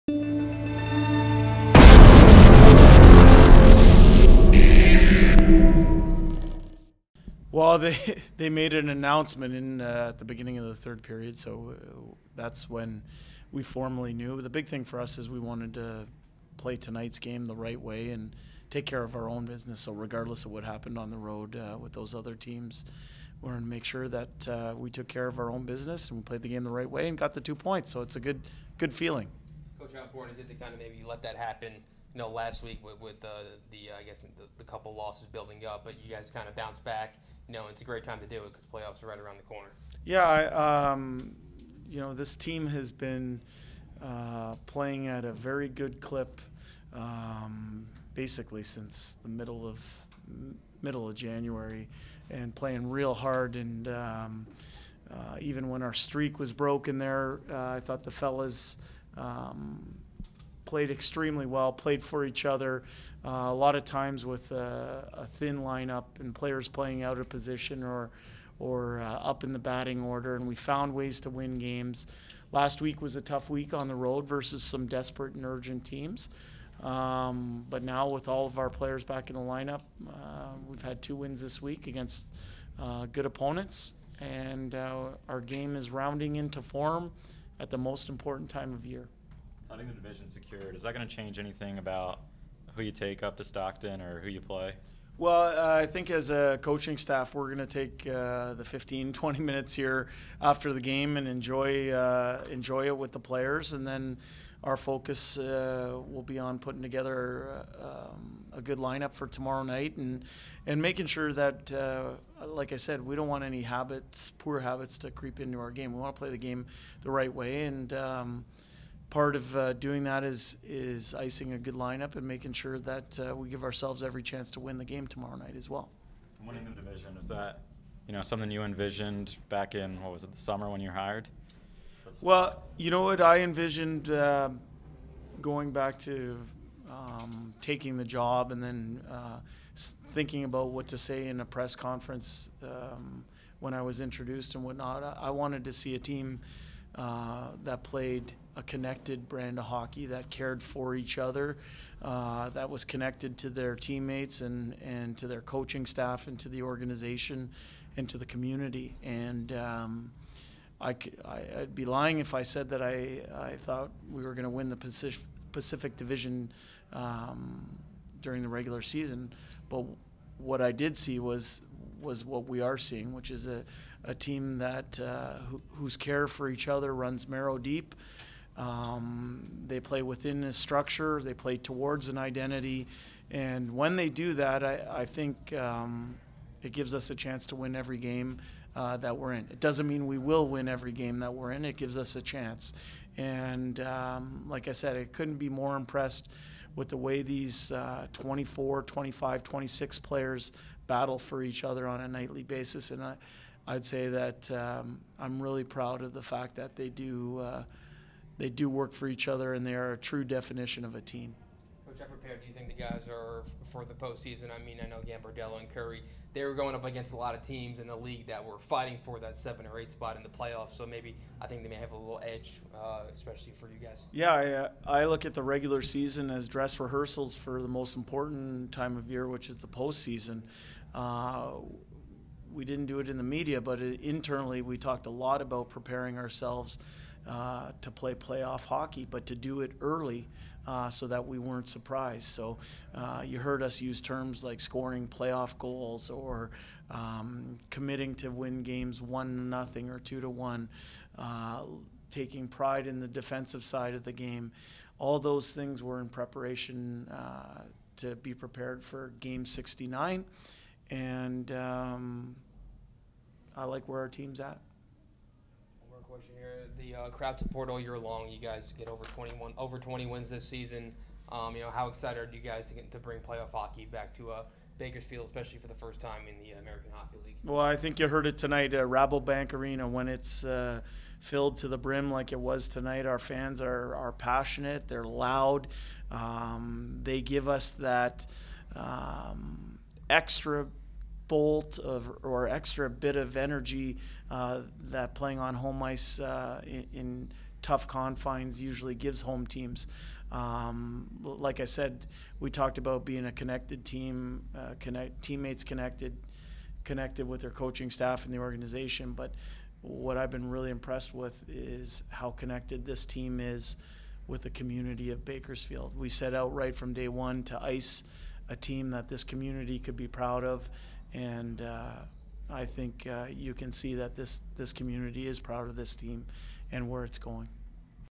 POST-GAME: Woodcroft on 6-3 win
Post-game comments from Head Coach, Jay Woodcroft, following 6-3  win against Ontario.